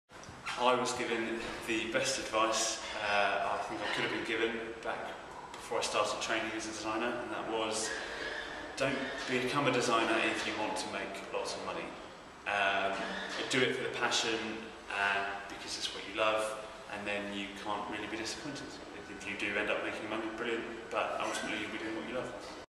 What key piece of advice do you wish you had been given...? Chris Brooker
Design Wales asked product designer Chris Brooker, What key piece of advice do you wish you had been given before you started your creative career?